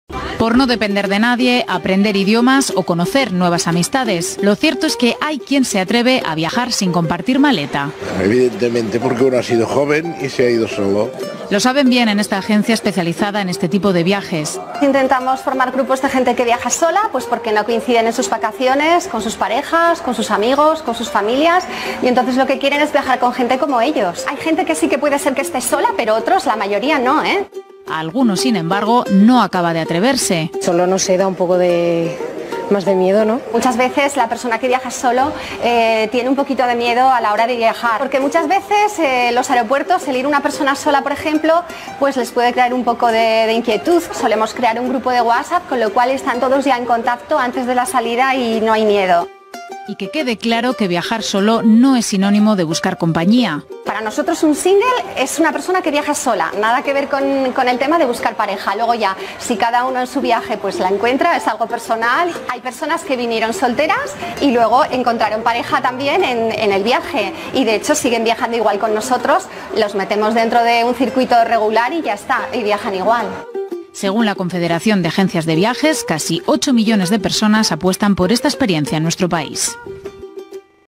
VIAJAR SOLO Agencia de Viajes
Viajarsolo.com ofrece un valor añadido a esas personas que quieran viajar de tal forma. La agencia de viajes especializada en singles ofrece viajes por todo el mundo a través de grupos organizados.